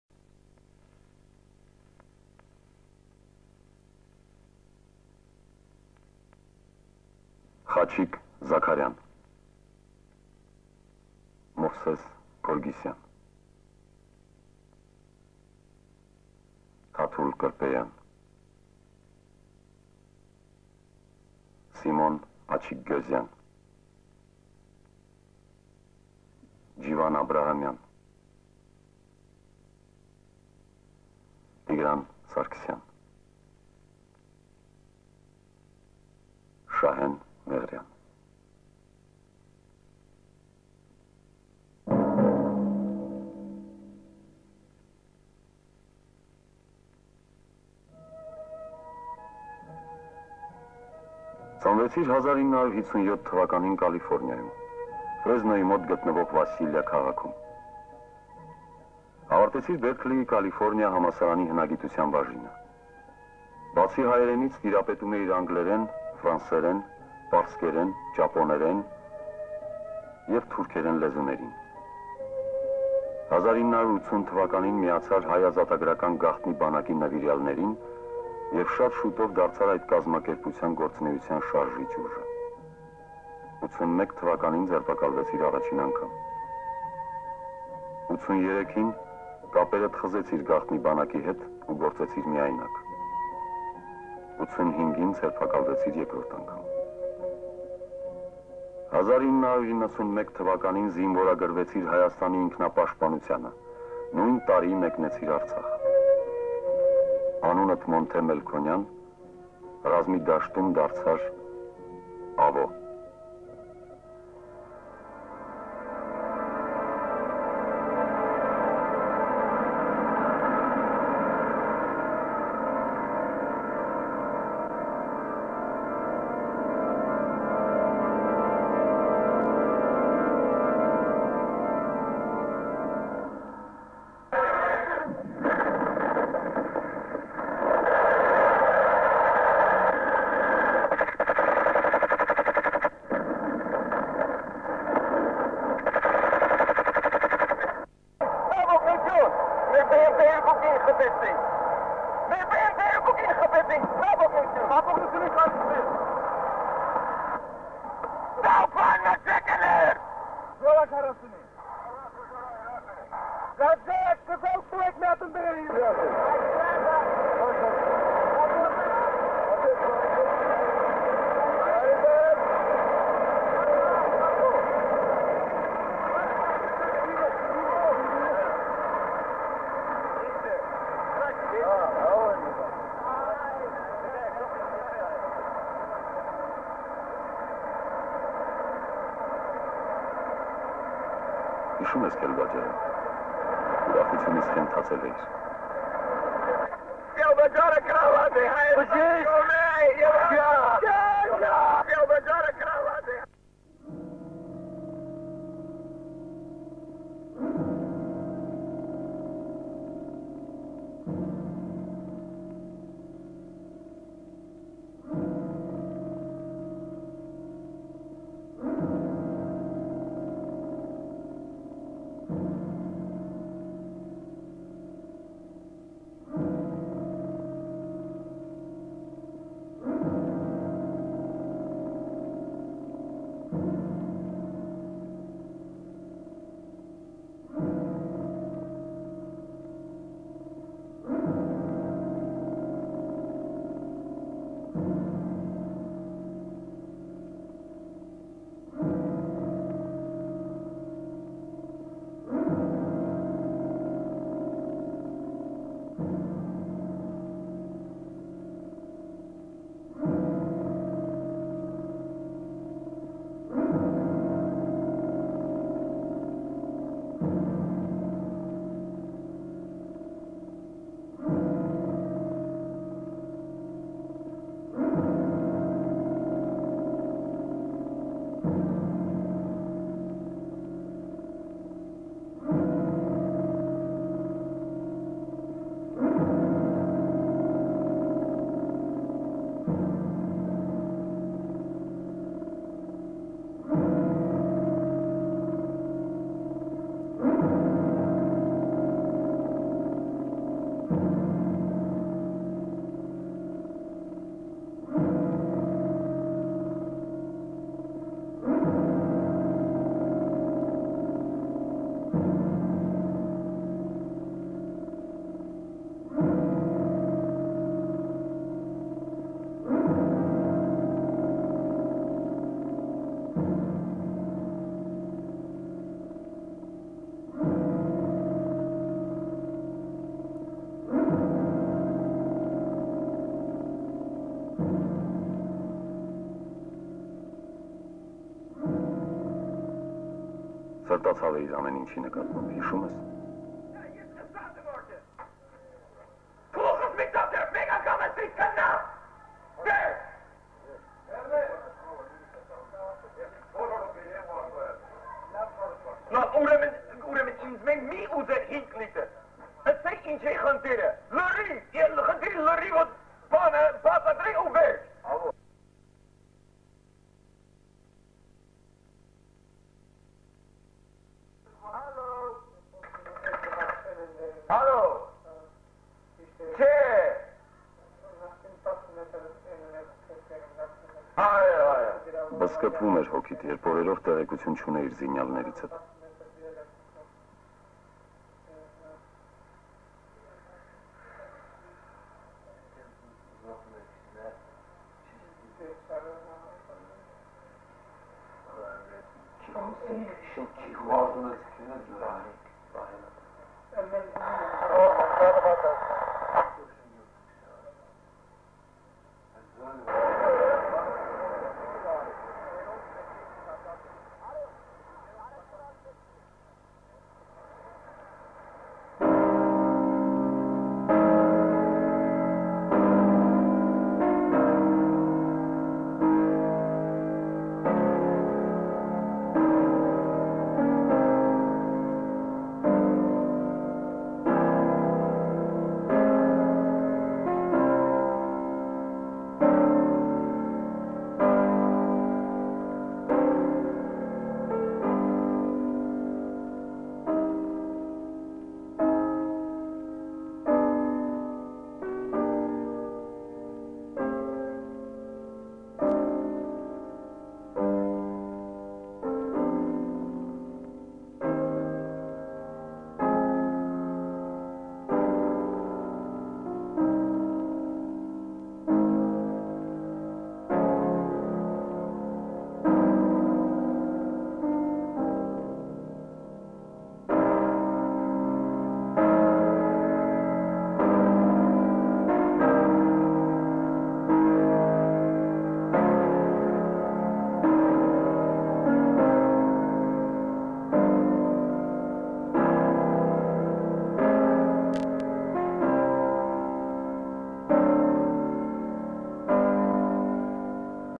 Monte Melkonyan vol. 1
Movie about Monte Melkonyan in Nagorno Karabakh. Part 1.